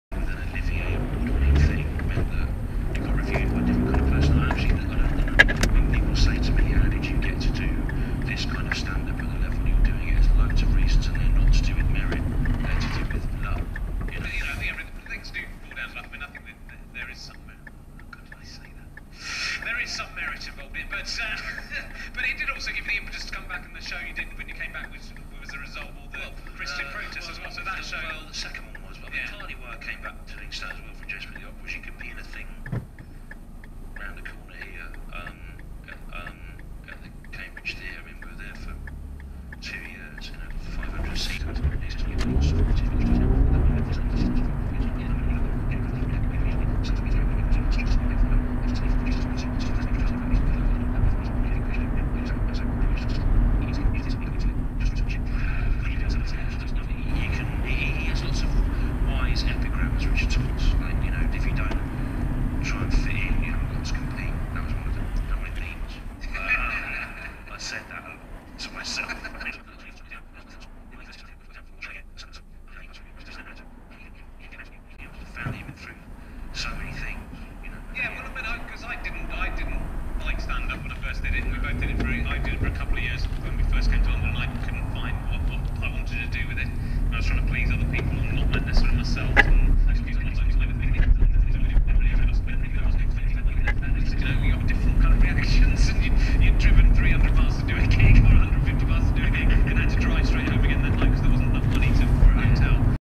VAN - HY17 POU
HY17 POU - Cuts up at the first roundabout. Then does it later on at the lights. If going into town he should be in that queue at the lights on the right, but he's not there. Instead he's back on the A27 at the end in the distance meaning he cut in at that the 2nd set of lights.

Knob.